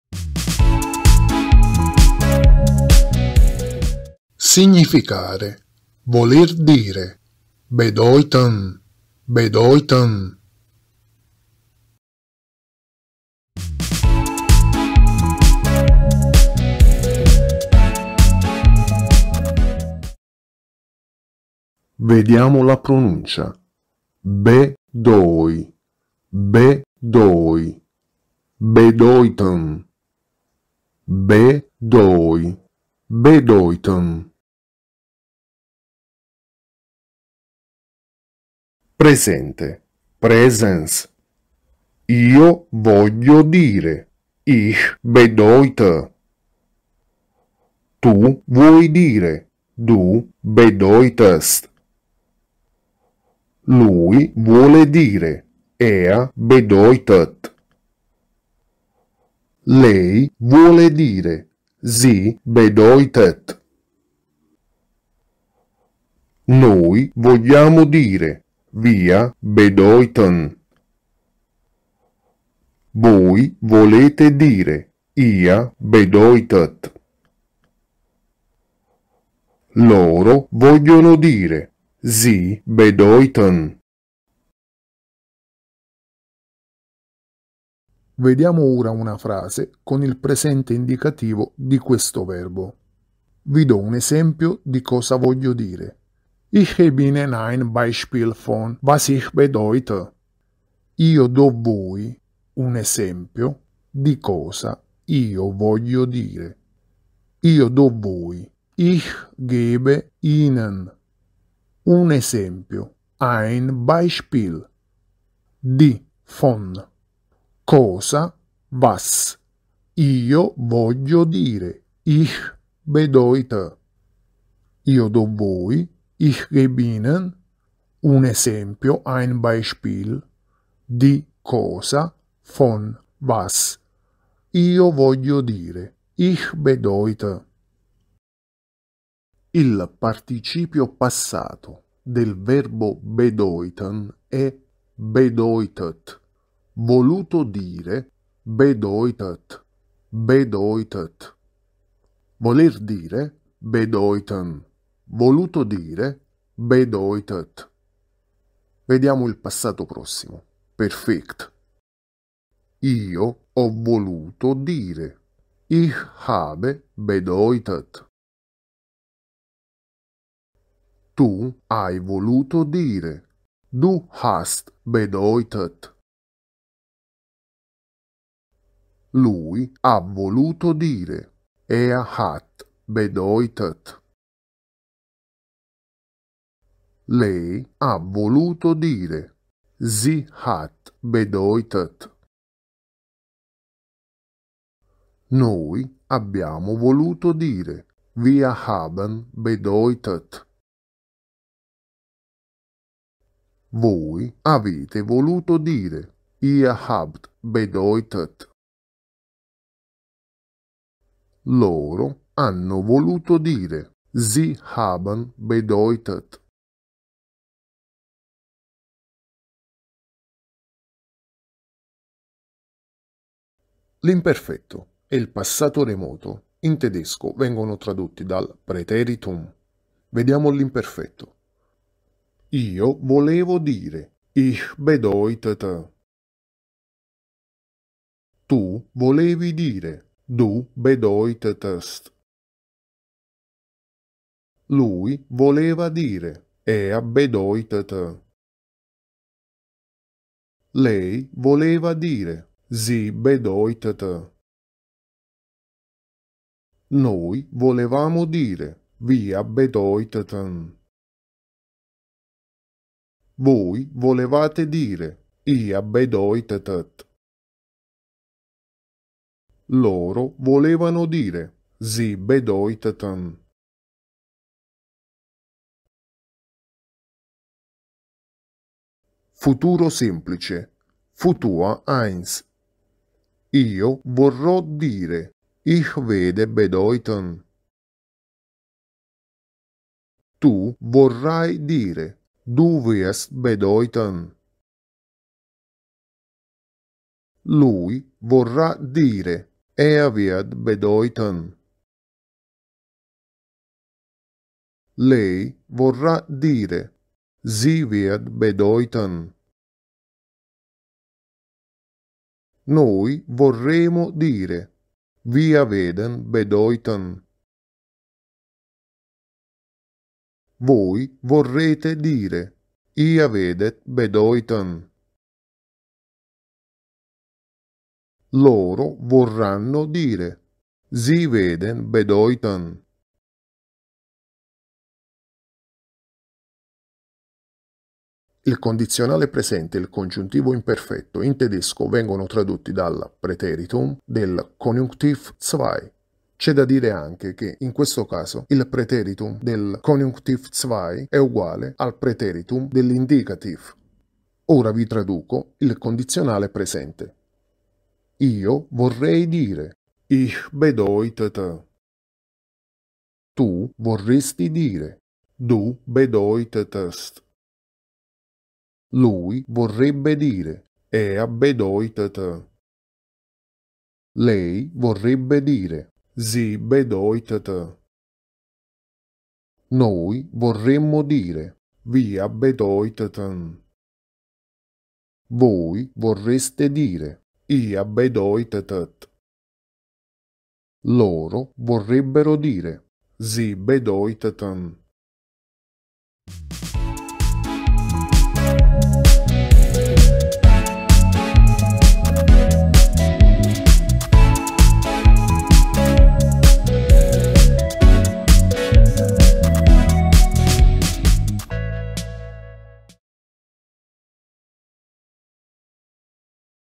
tedescofacile​ TEDESCO FACILE #262 -- IL PASSATO REMOTO
La coniugazione del verbo BEDEUTEN = -- voler dire -- significare